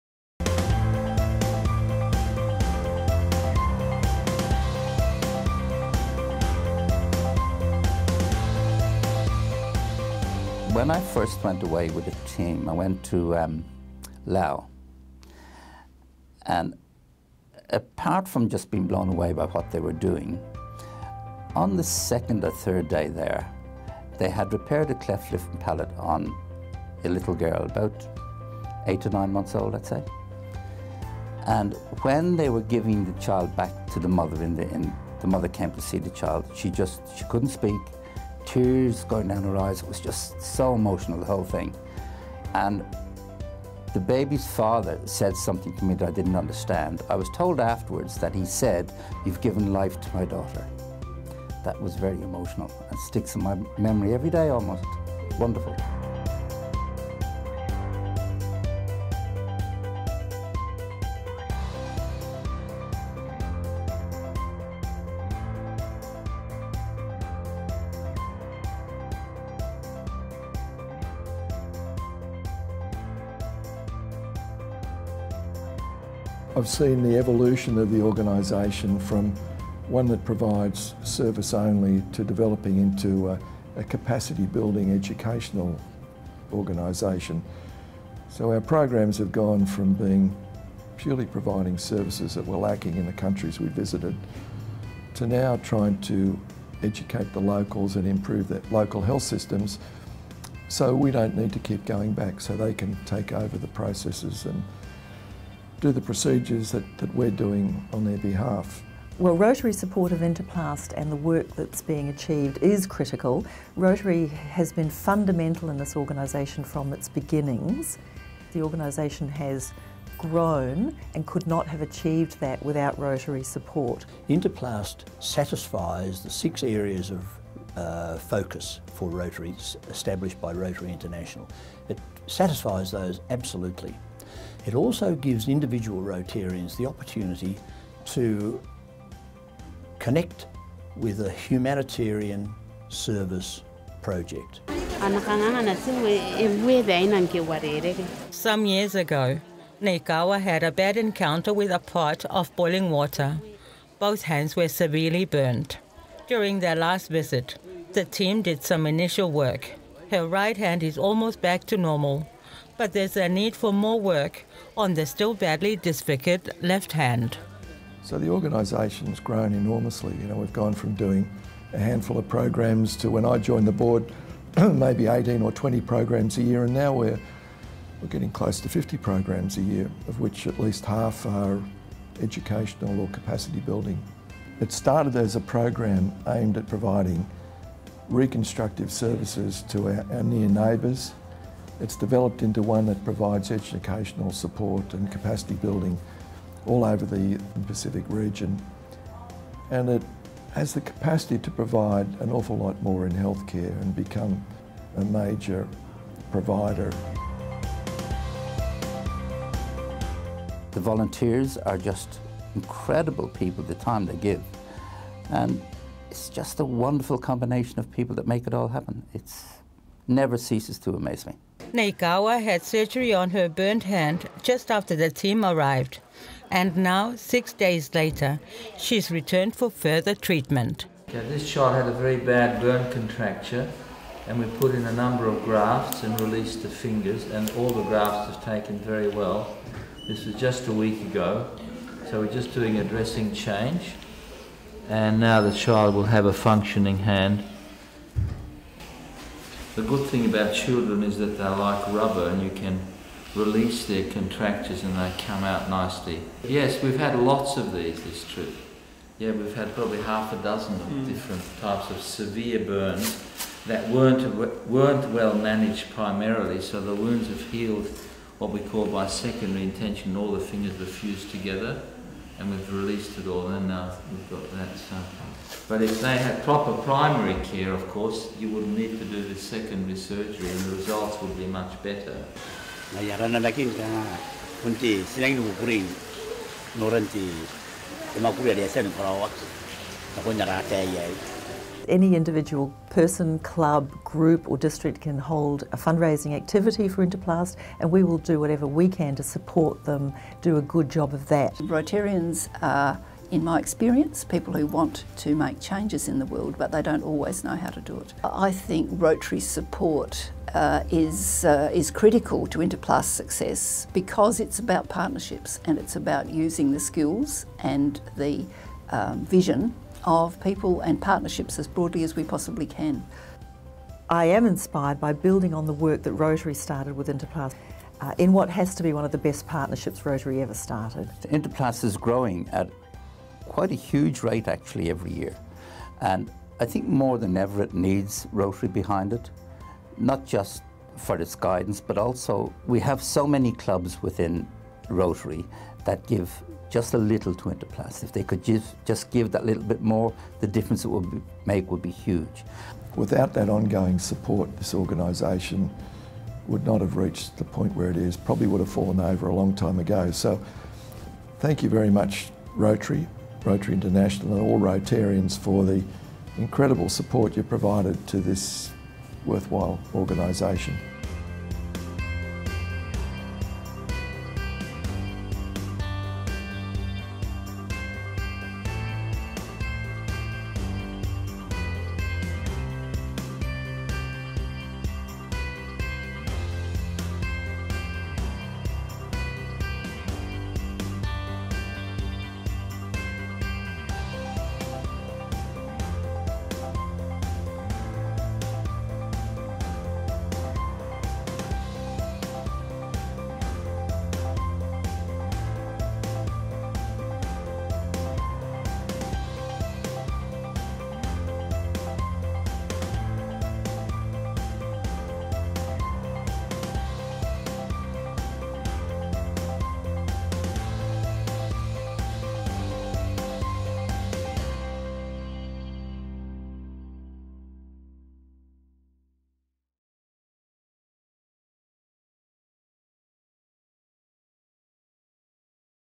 Interplast and Rotary - changing futures together across the Asia Pacific
For more than 30 years Interplast Australia and New Zealand has been sending volunteer teams to repair bodies and rebuild lives across the Asia Pacific.

What started as a partnership between Rotary and the Royal Australasian College of Surgeons has become a life-changing organisation working in 17 developing countries in our region.

Find out more about this great work and how Rotary continues to be an essential supporter.